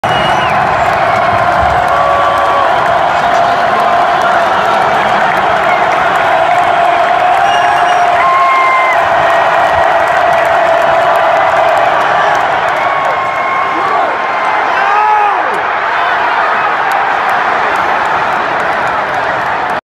Justin Tucker's 61yard game winning field goal
Justin Tucker hit a 61 yard field goal and defeated the Detroit Lion's 18-16